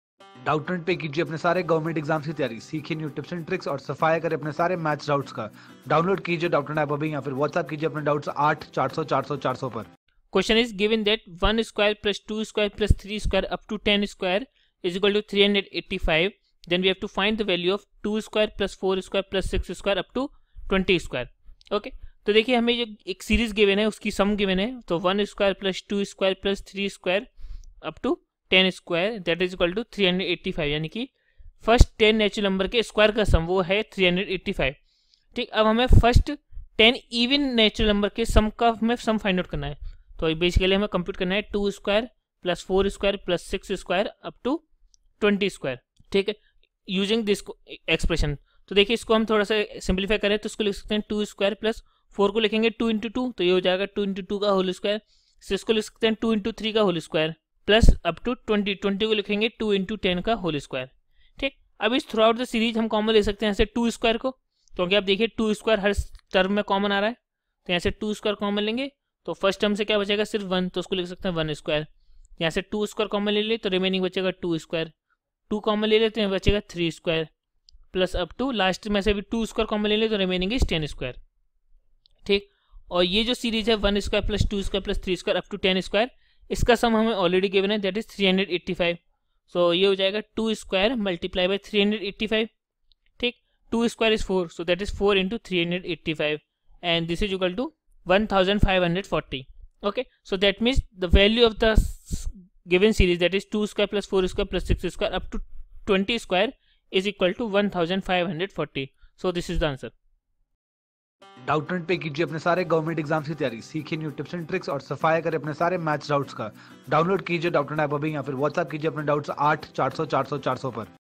Given that `1^2+2^2+3^2+dot\\ +10^2=385 ,\\ ` then find the value of `2^2+4^2+6^2+\\ dot\\ +20^2dot`
Question From -  Quantitative Aptitude By RS Aggarwal Chapter 2 NUMBER SYSTEM  for  Bank PO,SBI PO, IBPS PO,SSC, Railway Entrance & RBI Exam.